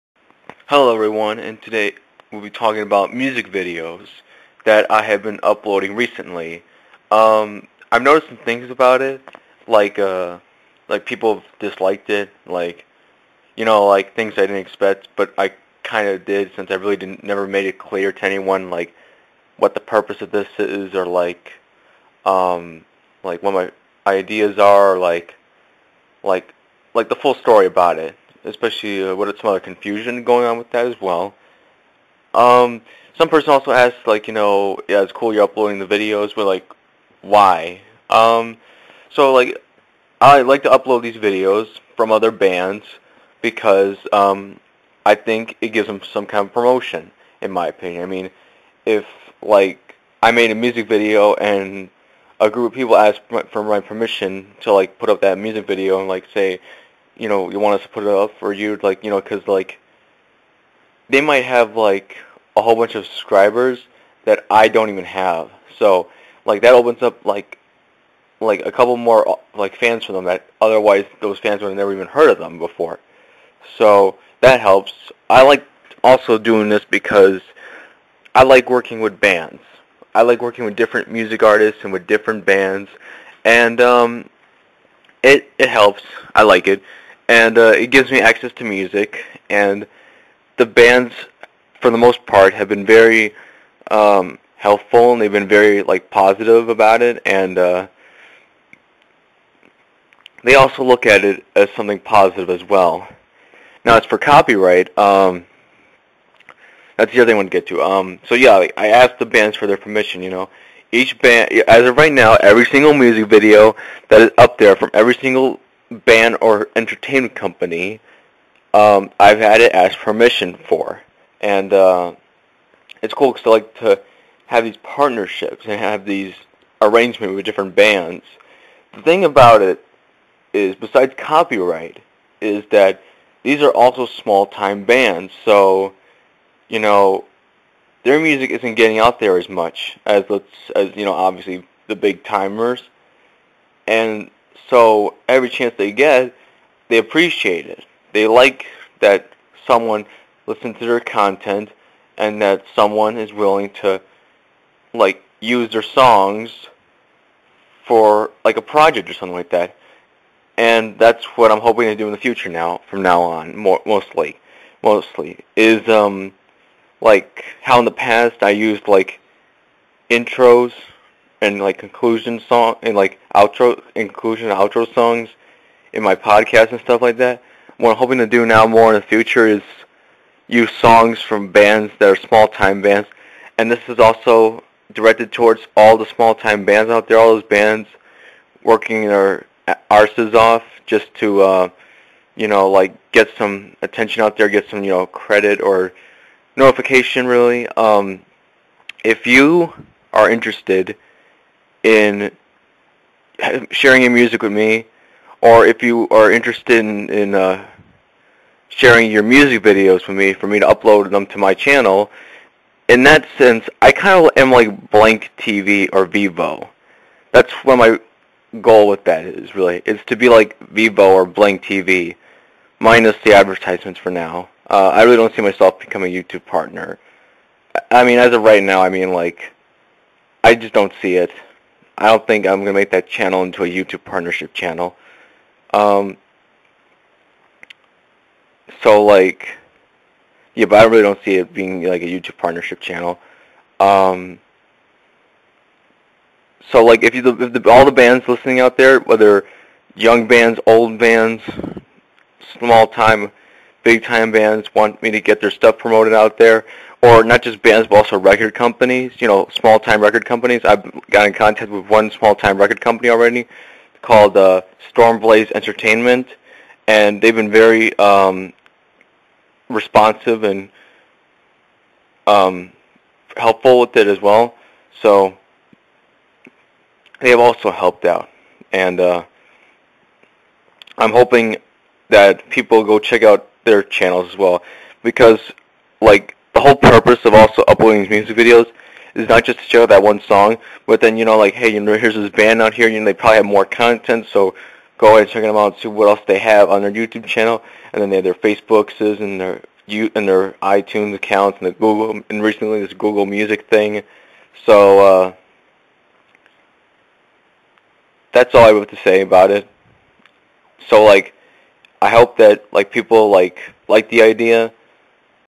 Music Videos- Rock & Metal Disciples podcast
I've been uploading music videos recently from different in this podcast I talk about it also if any bands watching want to share their music videos, or songs inbox me